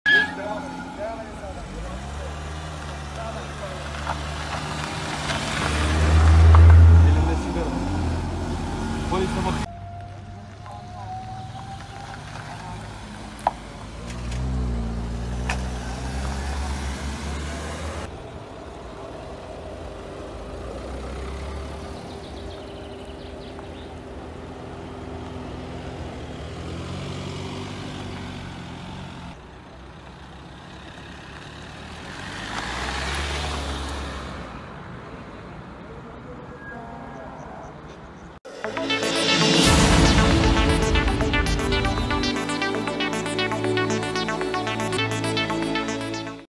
Kaynarca-Cebeci yolunda feci kaza: 4 yaralı
Sakarya’nın Kaynarca ilçesinde otomobil ve hafif ticari aracın çarpışması sonucu meydana gelen trafik kazasında 4 kişi yaralandı.